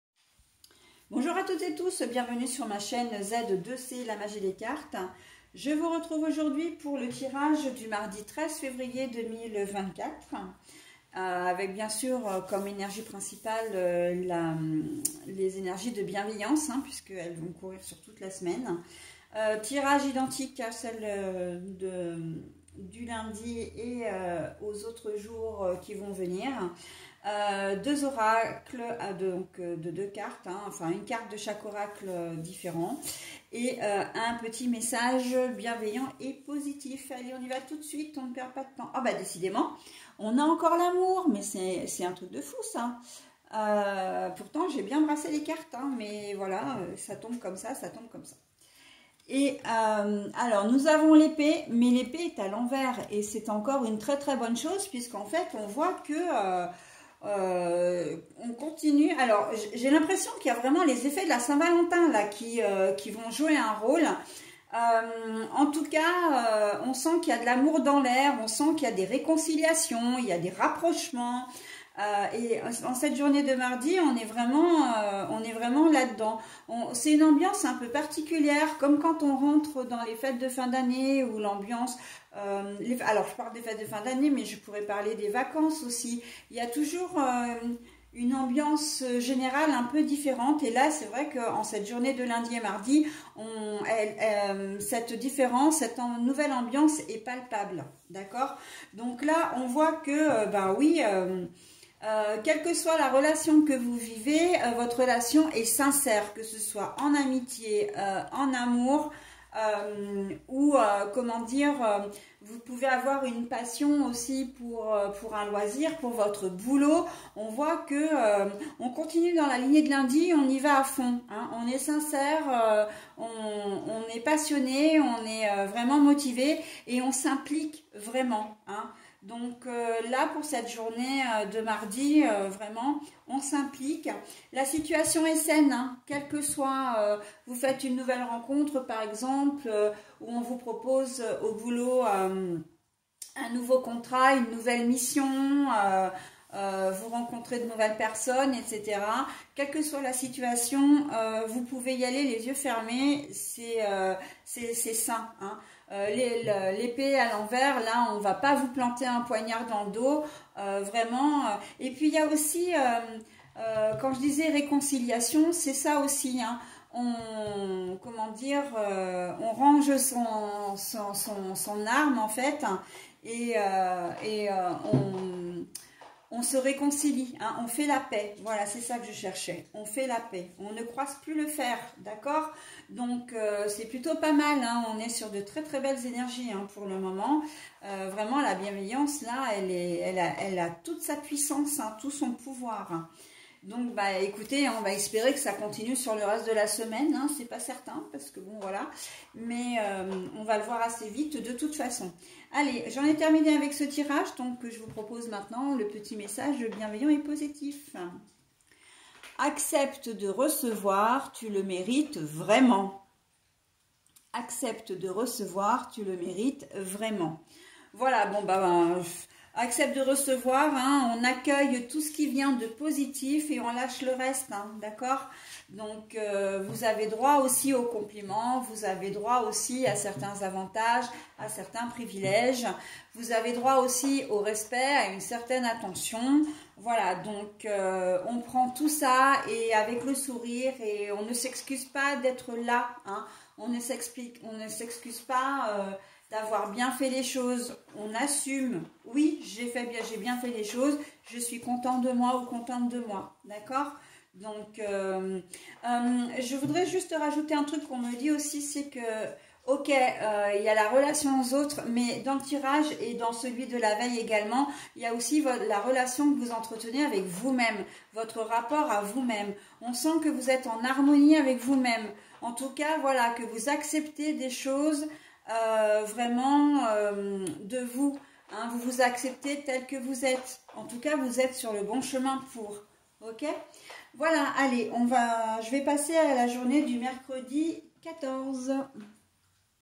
Mardi 13 février 2024 * Réconciliation/nouvelle rencontre. Situation saine.
Semainier de Z2C la Magie des Cartes
Energies principales : Oracle de l’Ombre et de la Lumière
Energies générales : Oracle Votre Destinée de Claude Alexis et l’Oracle Gé
Bonus : Le Petit Oracles des Murmures